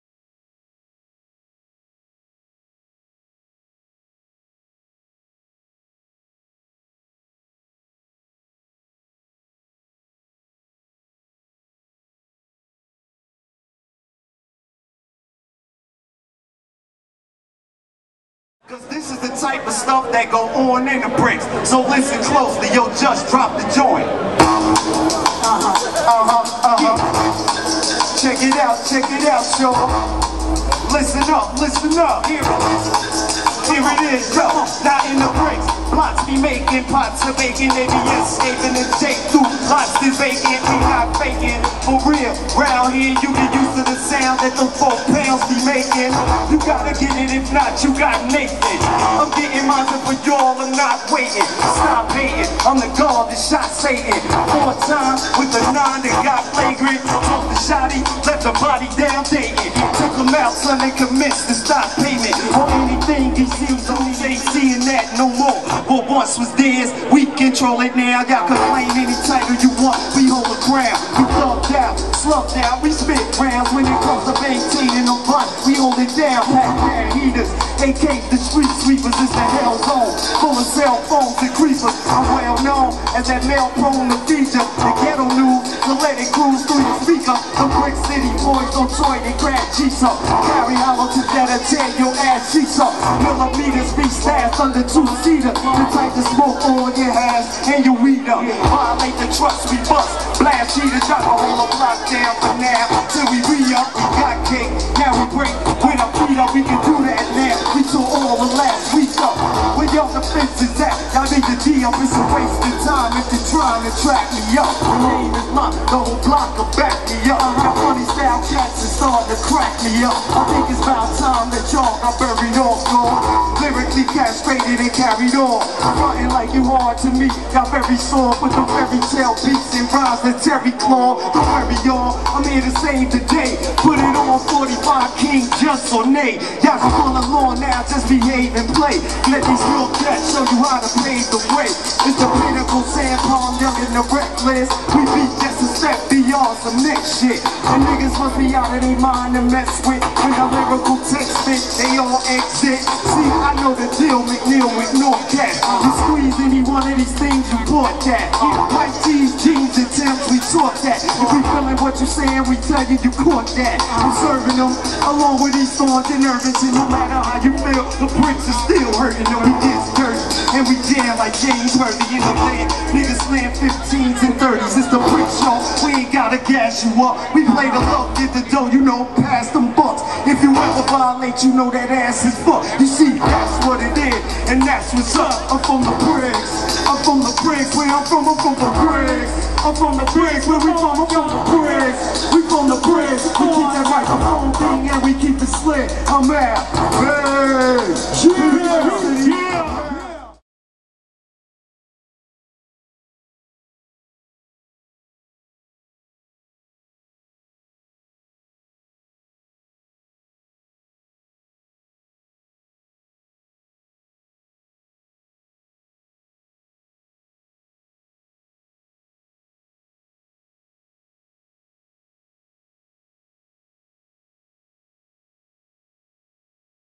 LAKIM SHABAZZ at Rock Steady Crew's 34th Anniversary.
Wily Reed Records presents live footage from FLAVA UNIT RECORDING ARTIST LAKIM SHABAZZ at Rock Steady Crew's 34th Anniversary in Newark, N.J., 2008.